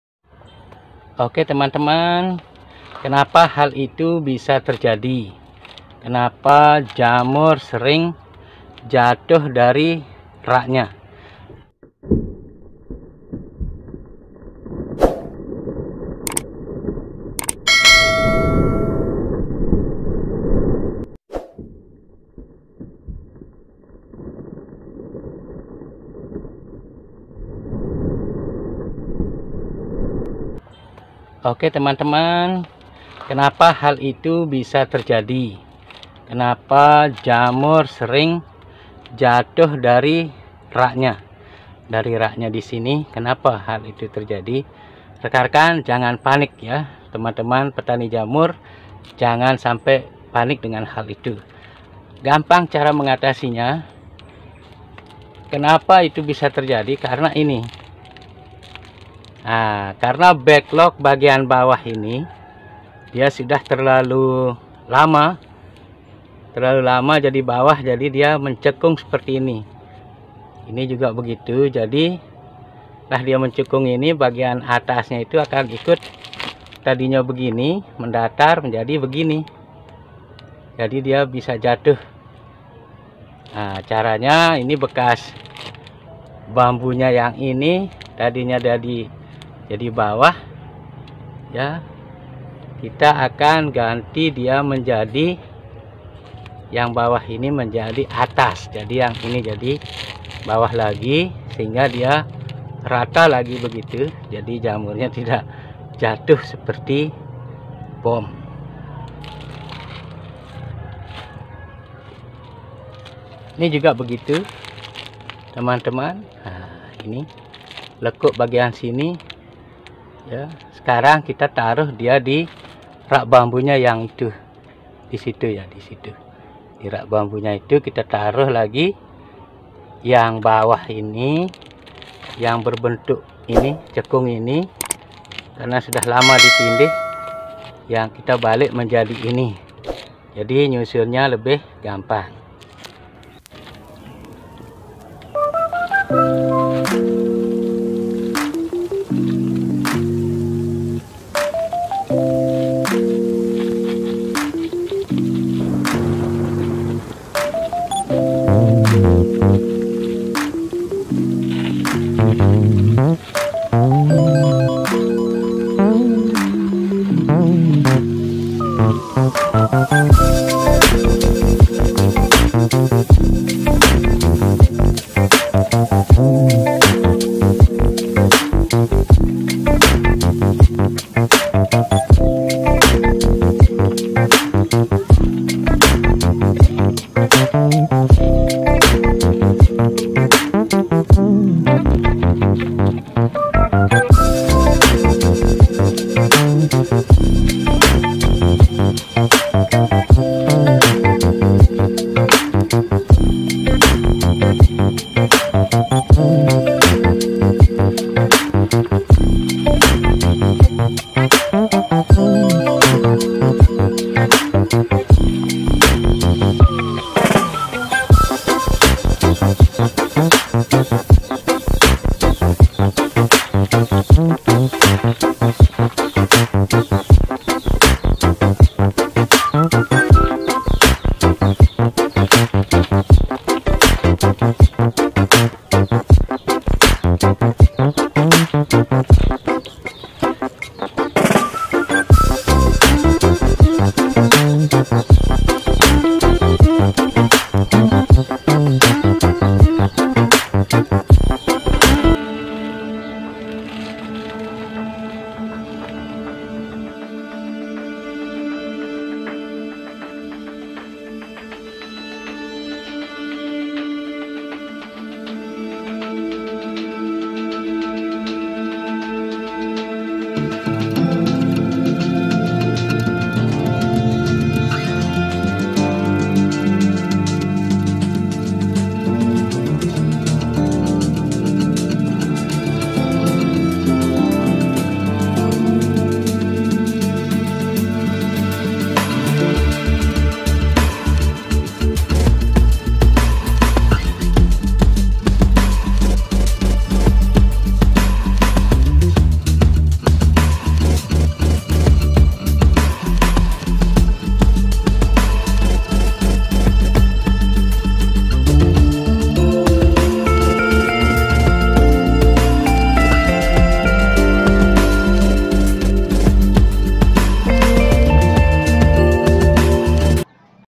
KENAPA BAGLOG JAMUR SERING AMBRUK DARI RAK ????berikut tips cara solusi terbaik.Oyster
banyak permasalahan dengan baglog jamur tiram seperti baglig ambruk dari rak bambu atau rak dari baja ringan. baglog juga banyak kontaminasi seperti kontaminasi jamur lain bahkan ada baglog kontaminasi seperti bubur.baglog jamur tiram terbaik adalah yang berwarna putih namun bukan putih seperti jeli.rak jamur tiram yang baik dari bahan yang lurus dan sejajar.
baglog jamur tiram kering walaupun baglog jamurnya jumbo namun tidak terlepas dari masalah ambruk.